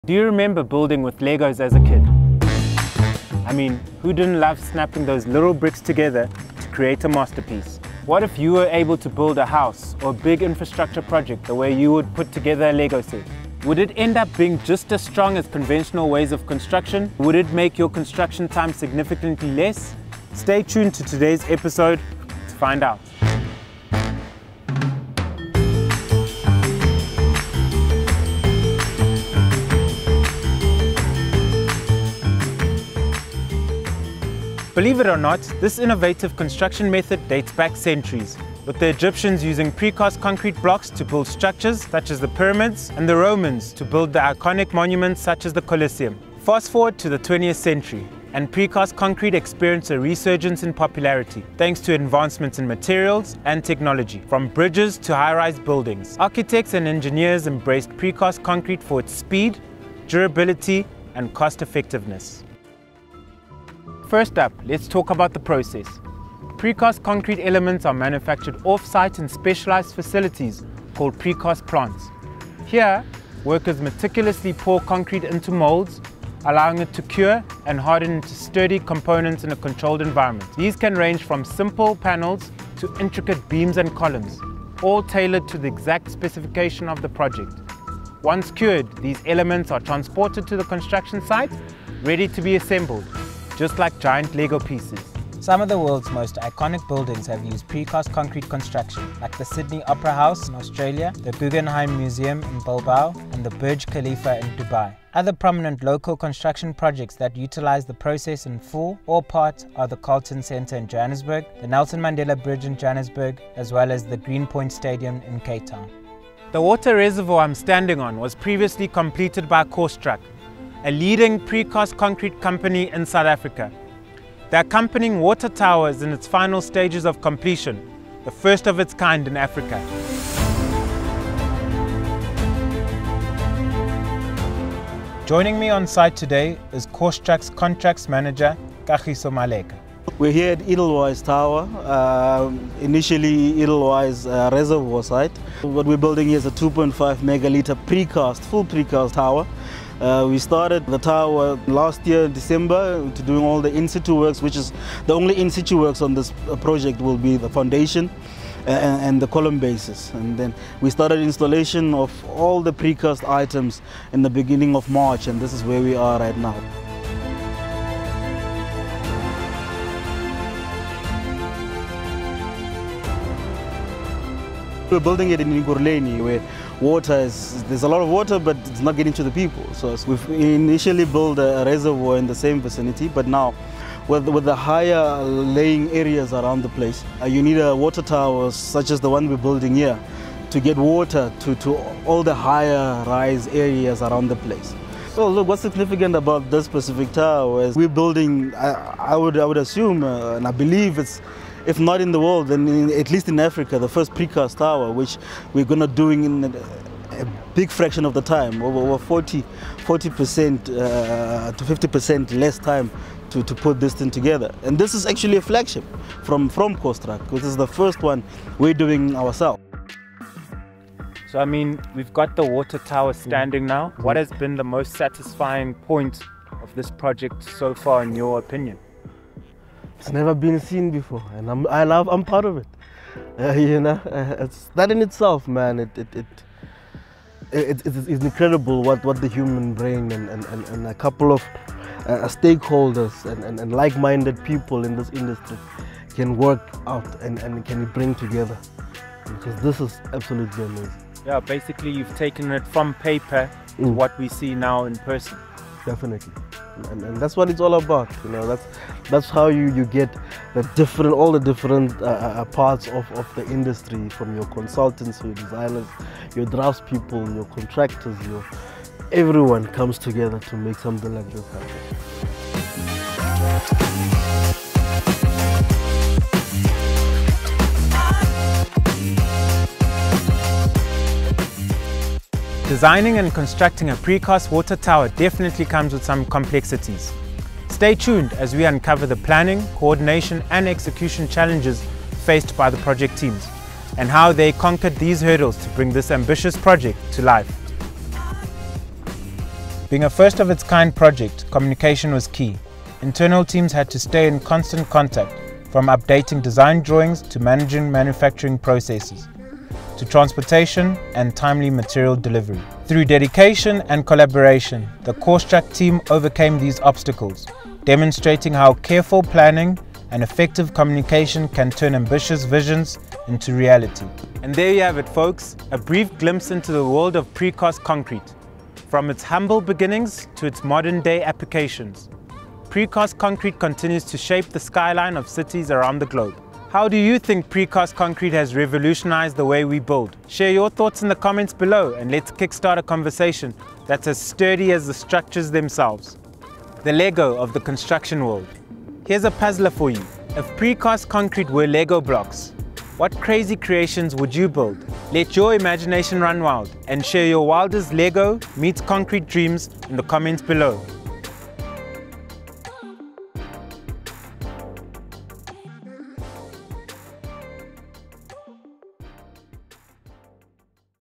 This 2.5 MILLION litre water tower was built in record-time! | Pre-cast Construction
This incredible feat of pre-cast concrete design, planning & construction by South African company Corestruc is a first of it's kind in Africa. Completed in less than 7 months, this water tower will be able to deliver water to high-lying areas in the Ekurhuleni local municipality in South Africa.